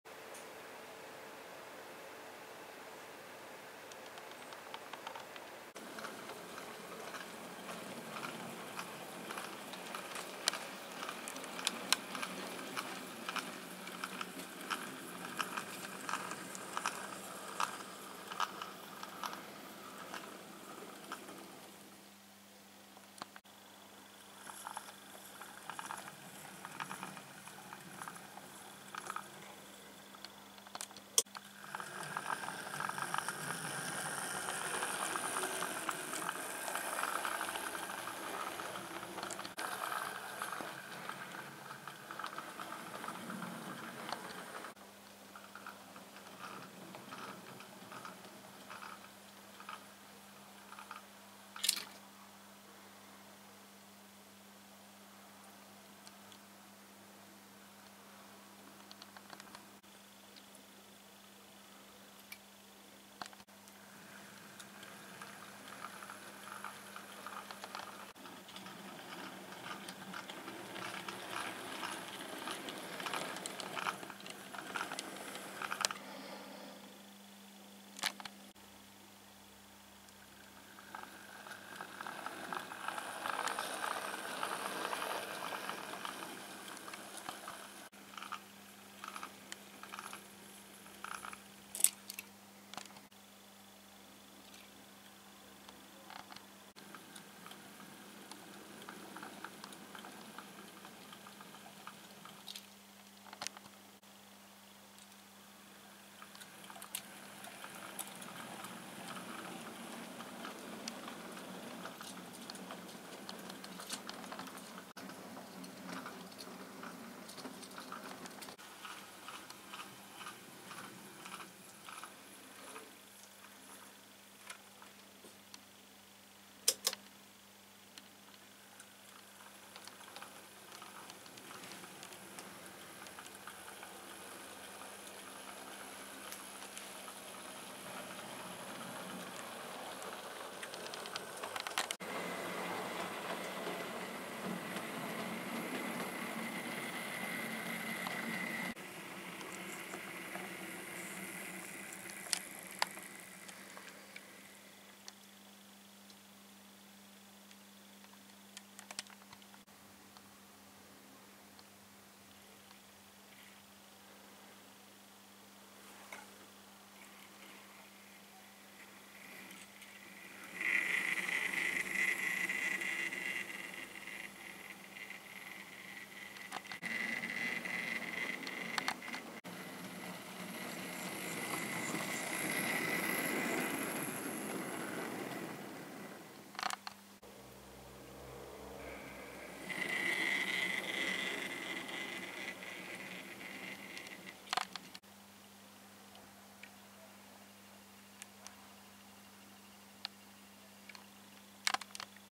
Mixed Trains
Running some mixed trains at Brockwell.
 Mixing passenger coaches and freight wagons is more efficient for the company but might mean passengers have to wait for wagons to be shunted before continuing there journey.